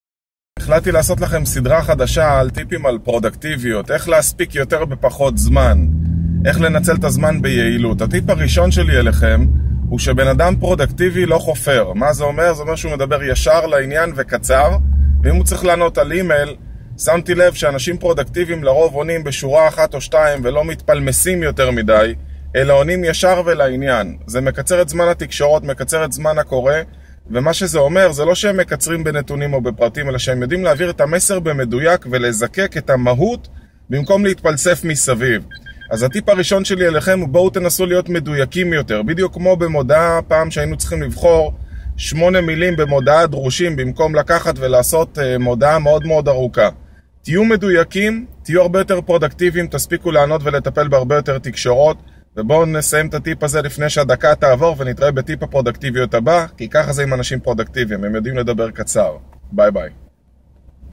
דברו קצר אל תחפרו
למה חשוב לקצר את התקשורות שלכם?
איך זה מקל על החיים שלכם ושל הסובבים אתכם?
ואיך זה קשור לפרודוקטיביות?
אלעד הדר, מחברת SUCCESS ייעוץ עסקי, עם טיפ נוסף מסדרת הפרודוקטיביות.